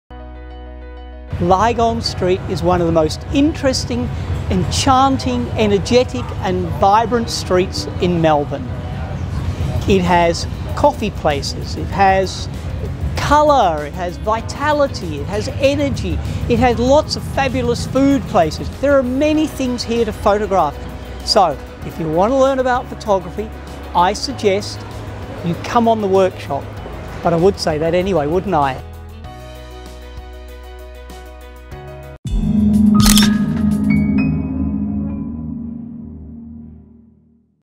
Lygon Street Workshop with Michael Coyne
Join our Lygon Street Workshop where you will photograph one of Melbourne’s most iconic streets post COVID-19 – and learn street and documentary photography techniques from Internationally renowned photojournalist and PSC Senior Fellow Michael Coyne!

The 2.5 day workshop will start on Thursday 10 December 2020.

Register now!